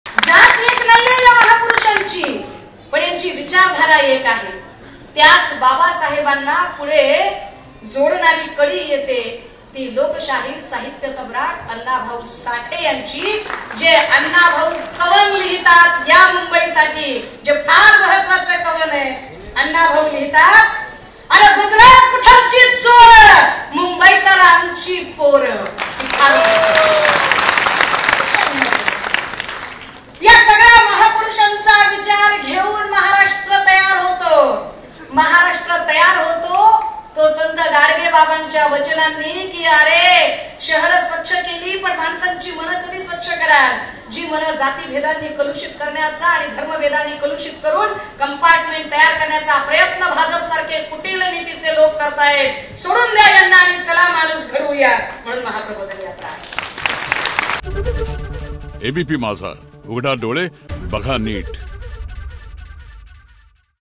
Sushma Andhare On annabhau sathe : महापुरुषांची जात नाही, मात्र विचार एक आहे - सुषमा अंधारे
Shushma Andhare On annabhau sathe : महापुरुषांची जात नाही, मात्र विचार एक आहे - सुषमा अंधारे